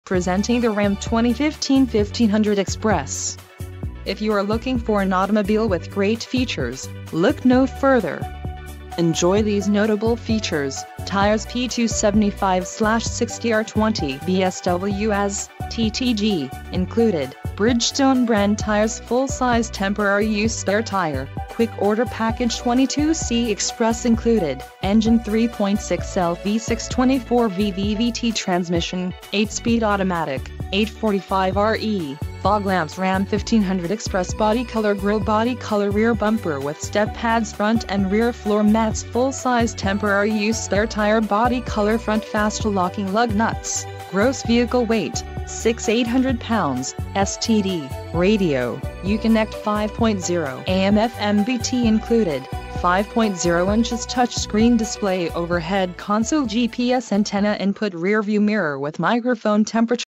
2015 Ram 1500 Express
CARFAX One-Owner. Granite Crystal Metallic Clearcoat 2015 Ram 1500 Express RWD 8-Speed Automatic 3.6L V6 24V VVT One Owner ! Accident Free, 20 x 8.0 Aluminum Wheels, 3.21 Rear Axle Ratio, 40/20/40 Split Bench Seat, 4-Wheel Disc Brakes, 6 Speakers, ABS brakes, Active Grille Shutters, Air Conditioning, AM/FM radio, Black Exterior Mirrors, Body Color Front Fascia, Body Color Grille, Body Color Rear Bumper w/Step Pads, Brake assist, Carpet Floor Covering, Delay-off headlights, Delete Class IV Receiver Hitch, Delete Spray In Bedliner, Driver door bin, Dual front impact airbags, Dual front side impact airbags, Electronic Shift, Electronic Stability Control, Exterior Mirrors w/Heating Element, Fog Lamps, Front and Rear Floor Mats, Front anti-roll bar, Front Armrest w/Three Cupholders, Front Center Armrest, Front wheel independent s... read more